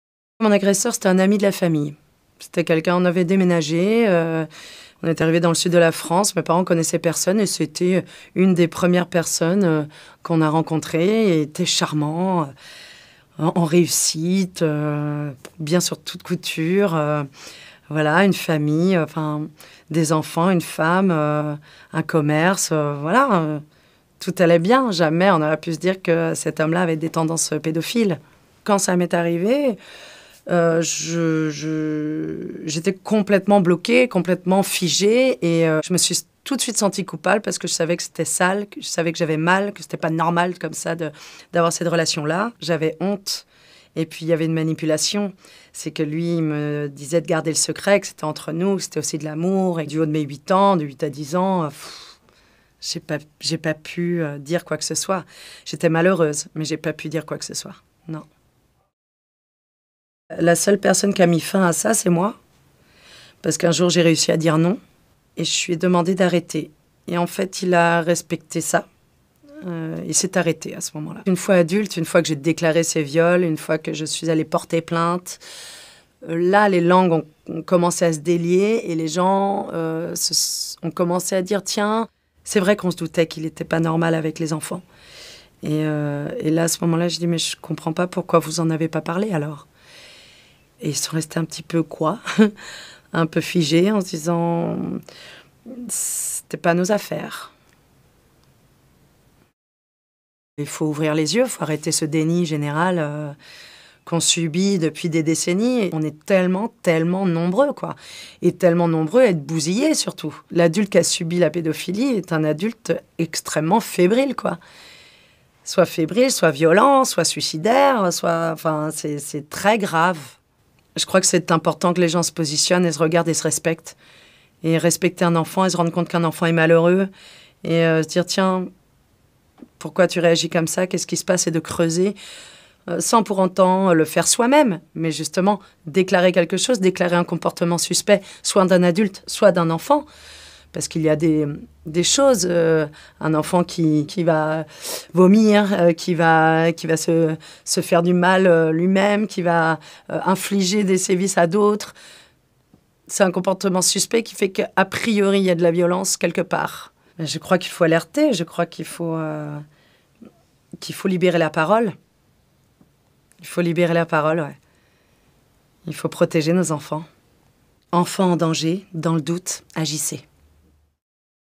Andréa Bescond - Témoignage - Campagne "Enfants en danger, dans le doute : agissez !" - 2017
Dans le cadre de la campagne "Enfants en danger, dans le doute : agissez !", Andréa Bescond, danseuse, auteure et interprète du spectacle « Les Chatouilles » nous livre son témoignage.

Vidéo mise en ligne sur la chaine du ministère Egalité Femmes Hommes le 1er mars 2017